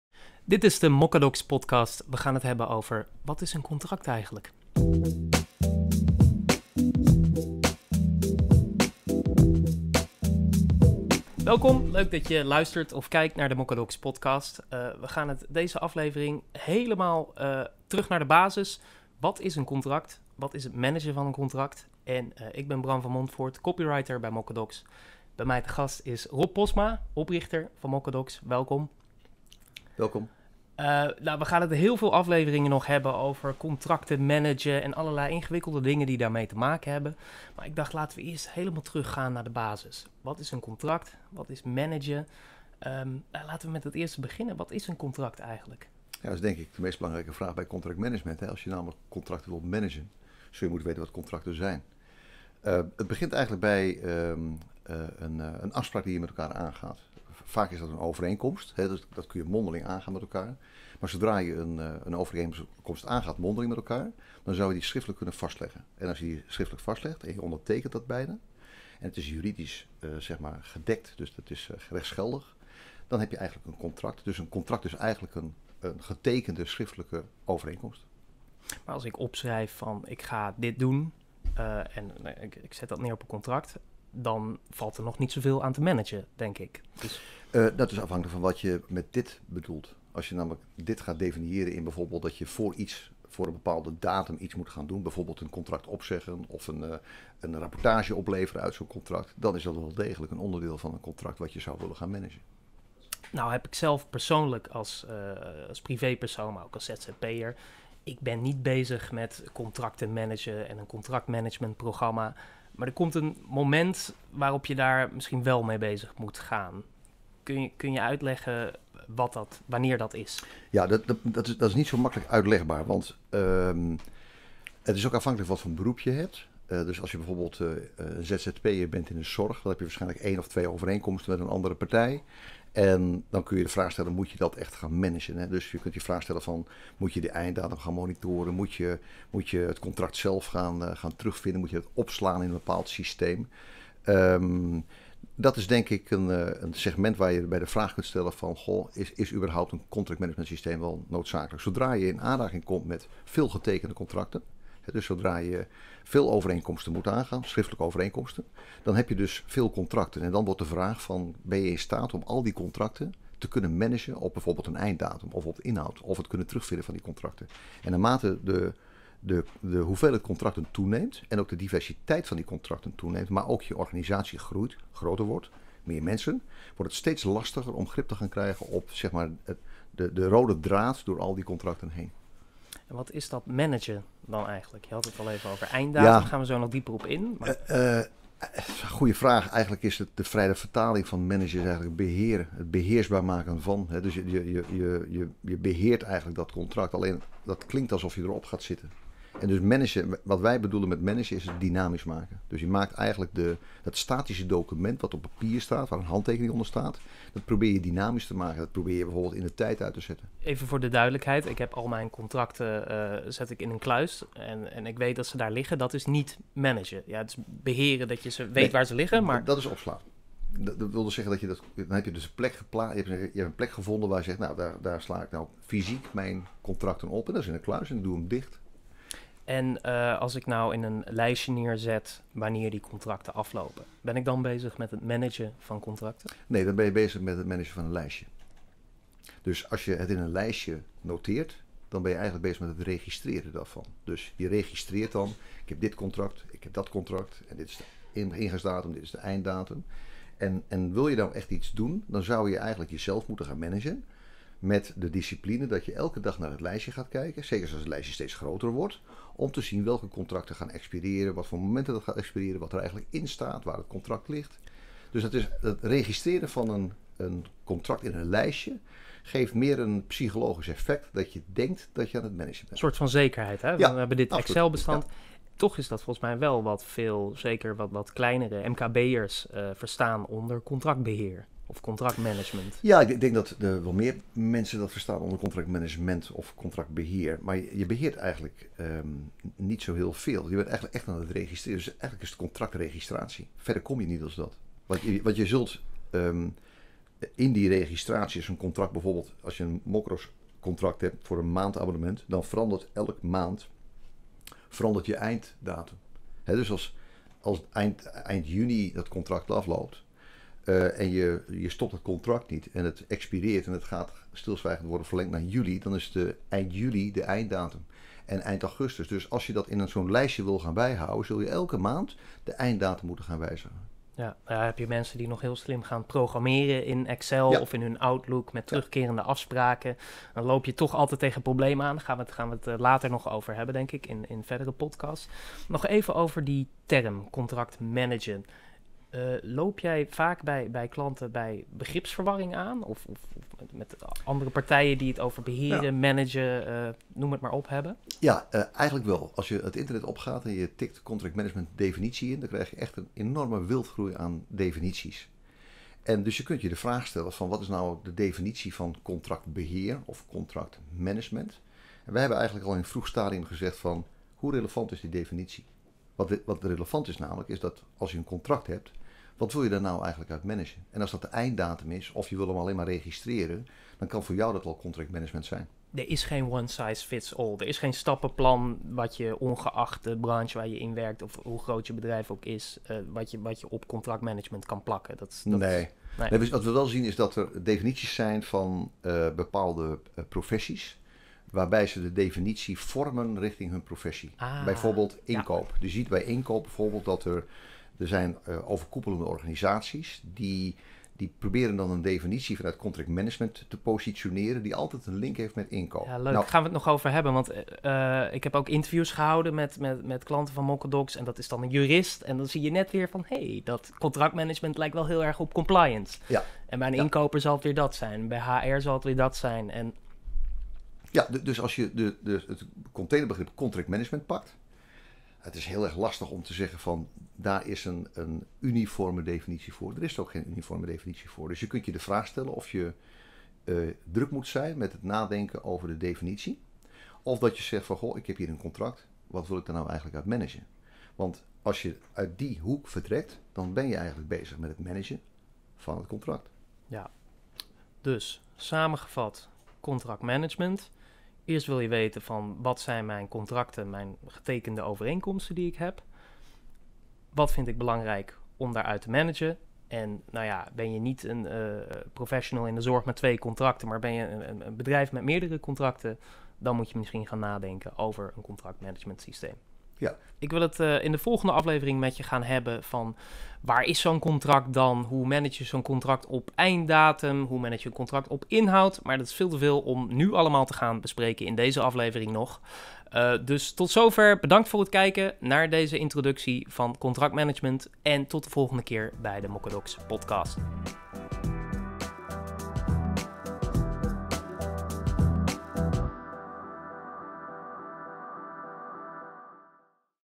Wat is een contract? - Podcast #016 | Mochadocs
Mochadocs - Create, Sign and Manage

Sommige vragen lijken zo simpel dat je ze vergeet te stellen.

'Wat is een contract?' 'Waarom moet je het managen?'

En toch is het belangrijk om er even over na te denken.

🎙 In deze podcast met Rob Postma & Bram van Montfoort gaan we terug naar de (hele nuttige) basis.

📝🤔 Wat is een contract?
Wij hebben in dit geval over afspraken die je schriftelijk vastlegt.

📝⏰ Wat is contractmanagement?
Je kunt al je contracten netjes in een kluis opslaan, maar dat is nog geen contractmanagement. Dat is het opslaan van contracten.

 Je kunt al je contractafspraken netjes in een lijstje zetten. Maar dan moet je elke dag naar dat lijstje gaan kijken. Dat is het registreren van contracten.

Wat bedoelen we dan met contractmanagement?

Dat is een methode waarmee je zeker weet dat de juiste persoon op het juiste moment een berichtje krijgt  om actie te ondernemen op een contractafspraak die voor je organisatie belangrijk is.

Bijvoorbeeld om te beslissen of je het contract wilt verlengen of niet. Vaak gebruik je hier een contractmanagement oplossing voor.

👨‍⚕️👩‍⚕️/🏢 Moet iedereen contracten managen?
Als zzp'er in de zorg met twee opdrachtgevers kun je de vraag stellen of contractmanagement wel nodig is. Je kunt belangrijke afspraken zelf in je agenda zetten.

Wanneer de diversiteit en hoeveelheid contract toeneemt. is managen een must.

Wanneer je dit niet doet, loop je het risico dat contracten onnodig verlengd worden, stopgezet of je belangrijke afspraken niet uitvoert/ de andere partij belangrijke afspraken niet uitvoert.

🧐 Wat is de definitie van contractmanagement?
Het stikt op internet van de definitief voor contractmanagement. Wij denken dat er geen one size fits all definitie is. Een jurist zal met contractmanagement iets anders bedoelen dan een inkoper.

Hoe manage je de inhoud van een contract?